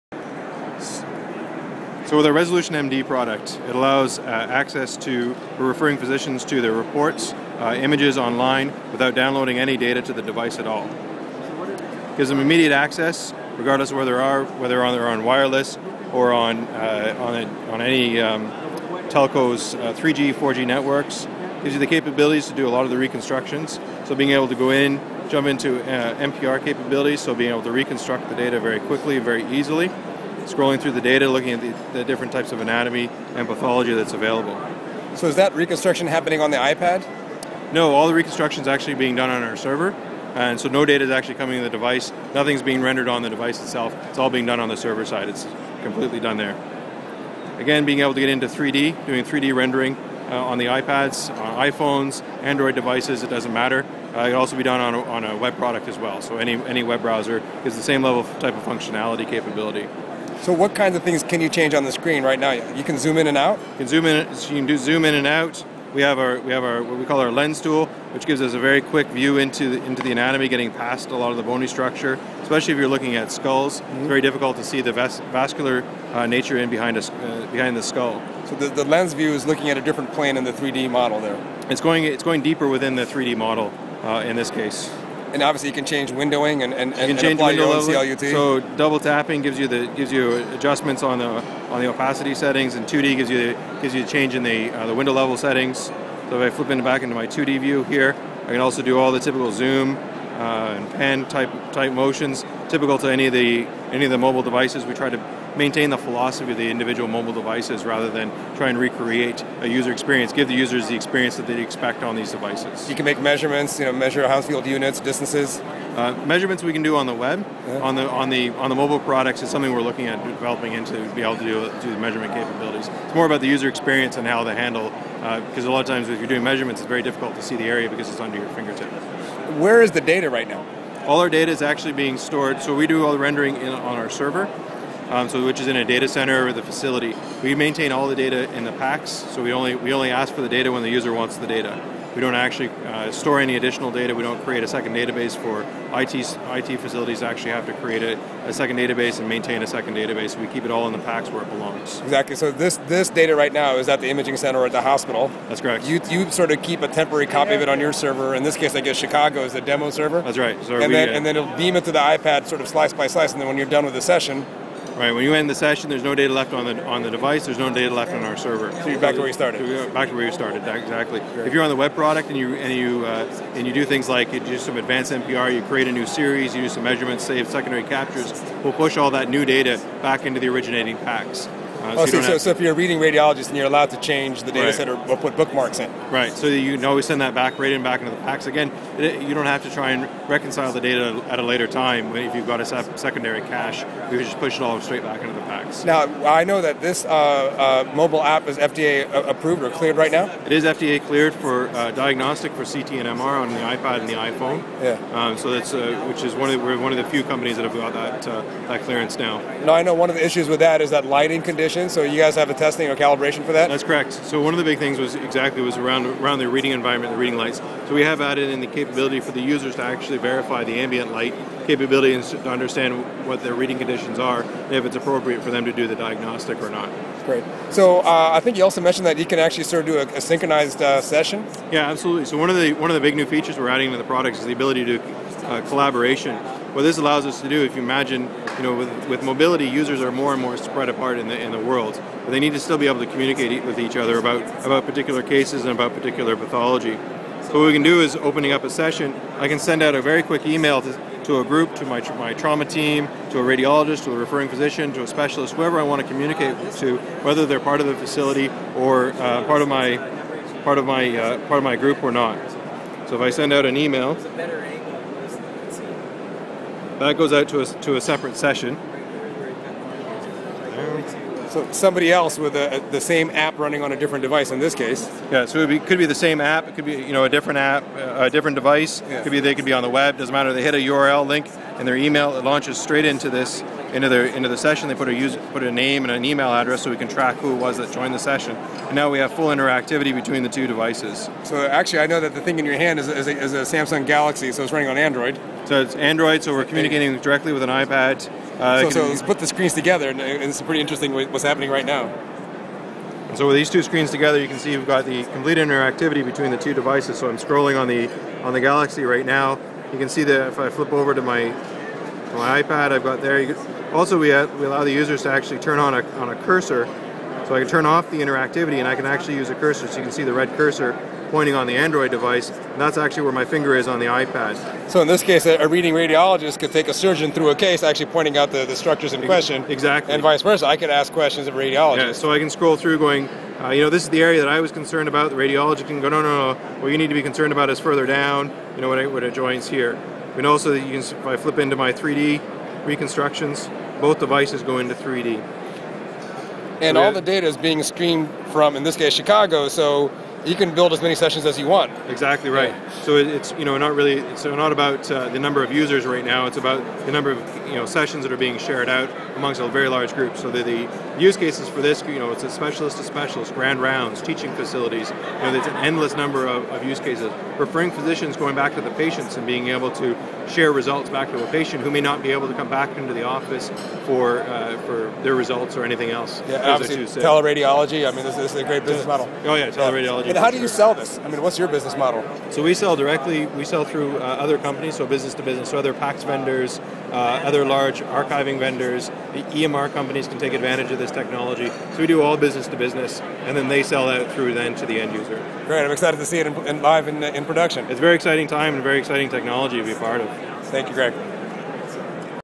ResolutionMD iPad radiology viewer
Demo of mobile app, mHealth Summit, 12/5/11